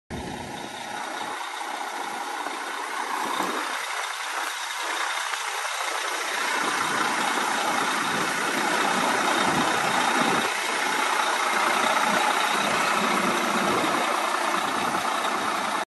2014 NATIONAL 1400H For Sale
Nice 2014 Freight liner 114SD with a National 1400H Crane mounted, the engine miles and hours would not show on dash please see the pictures, it did show 23,220 miles. Leverton Enterprises assumes those are the correct miles for the vehicle. Upon Inspection of this vehicle, Leverton Enterprises noticed the front drivers tire was flat, we aired it up and seemed to have a slow leak. Also the right side of the hood has damage ( customer had a blow out, please see pictures attached.) We could not get the Crane Level Therefore we couldn't get videos of the boom going out and back in but it is assumed to work. Please see all pictures and videos for all S/N plates, and all damages before hitting that bid button. Please give us a call with any questions on this lot or any other lots we may have coming up.